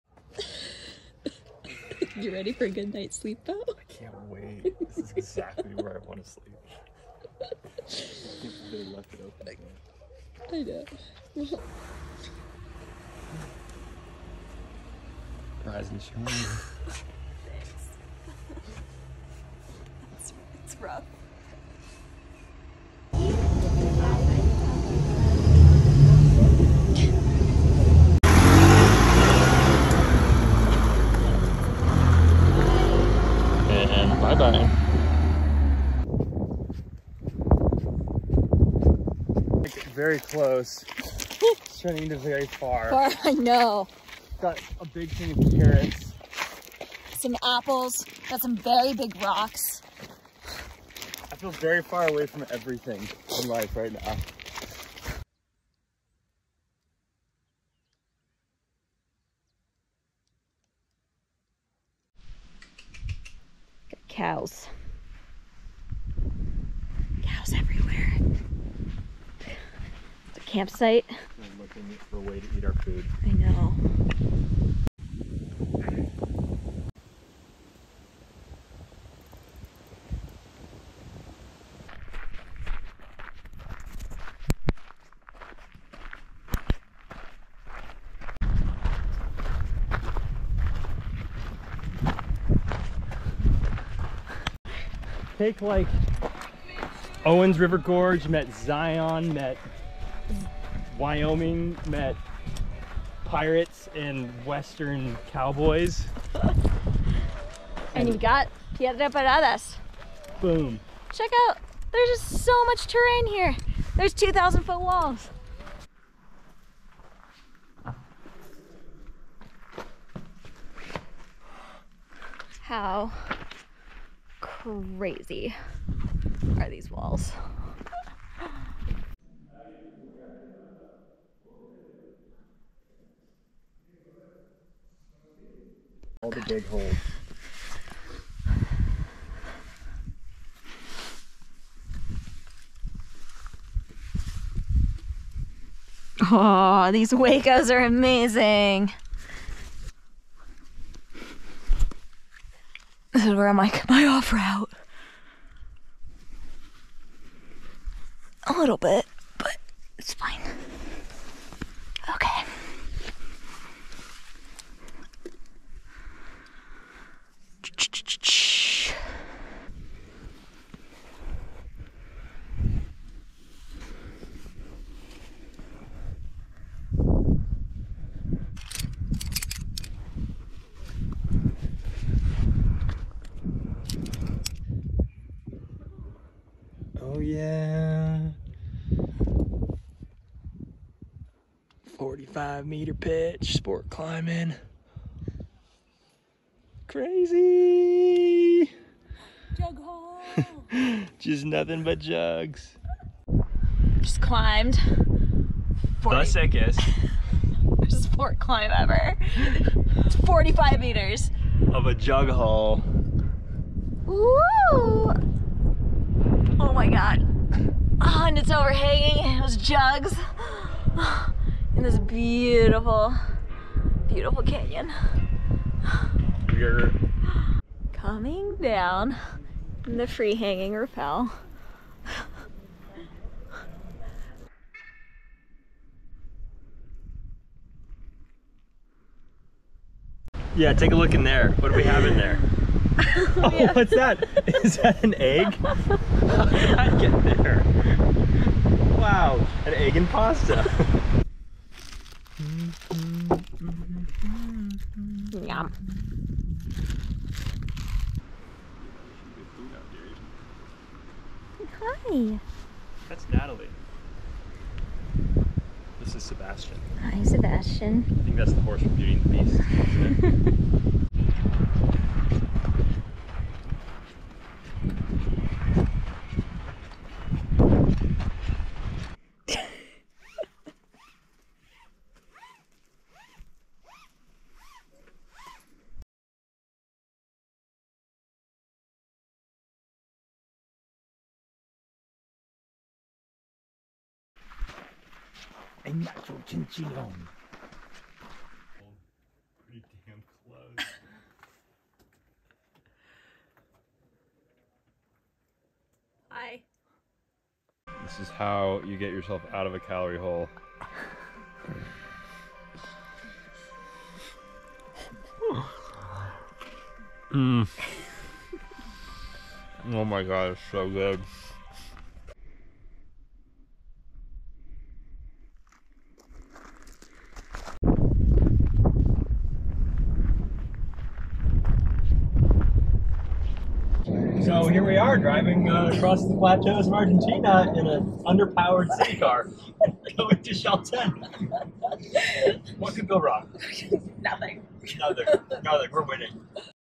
Piedra Parada Argentina Sport Climbing
Not wanting to spend weeks sitting around waiting for weather to clear up in El Chalten, Beau Skalley and I made the voyage to Piedra Parada: a sport climbing venue in the middle of nowhere in the deserts of Argentina with huge 2000' walls and stellar, varied climbing. As all adventures, we slept in the bushes behind bus terminals, had idyllic river camping, chased brazen cows from camp, climbed route after stellar mega-sport route and worked on perfecting our culinary Jet Boil skills. Highly recommend spending a week or two here if weather is iffy in the Torres!

[Note: I'm still learning to shoot horizontally with my phone/gopro, so apologies for the graininess of some of the clips at the beginning!]